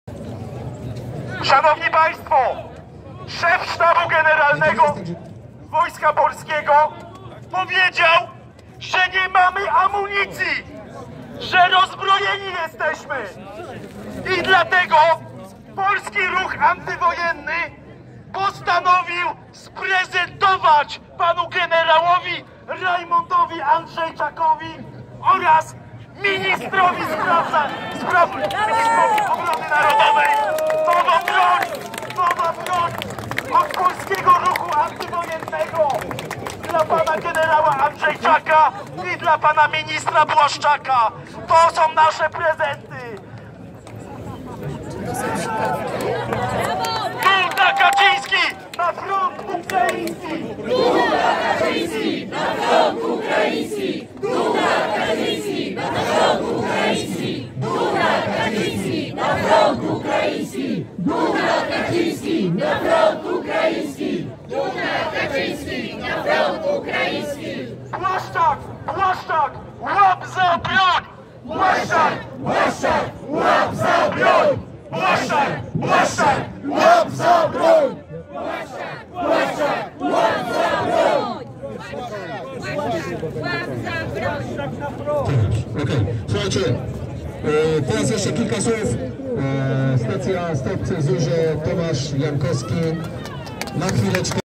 We are out of Ammunition- Rajmund Andrzejczak Polish Army General - says Leszek Sykulski, 1 May 2023
Marsz Pokoju, Anti War Rally in Warsaw ! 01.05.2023

 Thousands of demonstrators marched through the streets of Warsaw on Monday for a Labour Day march against the US and NATO warmongering, demanding an end to Poland’s involvement in the ongoing conflict in Ukraine.
 Crowds of protesters were seen with Polish flags and banners with messages such as, ‘This is not our war’ and ‘Yankee go home’ as they marched to the Castle Square in Warsaw’s Old Town.
"For over a year, we have been told by the authorities in Warsaw that our children are to go as cannon fodder, in the name of someone else's interests. That is why we want to show here that this is not our war, this is not our war," said Leszek Sykulski, founder of the Polish Anti-War Movement and organizer of the rally.
"There is no consent on our part to be dragged into the American war machine. We are saying here stop any imperialism, we are saying stop American aggression wars,” he added.
The protesters were demanding an end to Polish and NATO arms supplies to Kyiv and an immediate ceasefire in Ukraine. Polish people don't want to sponsor this war, to deliver weapons and. to die and kill for the interests of Anglosaxon hegemones.

"Od ponad roku władza w Warszawie wmawia nam, że nasze dzieci mają iść jako mięso armatnie, w imię cudzych interesów. Dlatego chcemy tutaj pokazać, że to nie jest nasza wojna" – mówił Sykulski, otwierając zgromadzenie. "My nie będziemy tutaj ważyć słów! My pamiętamy ludobójstwo dokonane na narodzie polskim przez nacjonalistów ukraińskich. Nie ma naszej zgody na symbolikę banderowską w miejscach publicznych. Nie ma naszej zgody na krzewienie kultu zbrodniarzy: Bandery, Suchewycza, Ukraińskiej Powstańczej Armii – ludobójczej organizacji ukraińskich nacjonalistów. Dzisiaj mówimy: Stop banderyzacji! Nie ma naszej zgody na brankę na cudze wojny, na wciąganie nas w amerykańską machinę wojenną! Mówimy stop jakimkolwiek imperializmom! Mówimy stop amerykańskim wojnom, stop tajnym więzieniom CIA w Europie, stop wpychaniu Polski w cudzą wojnę!"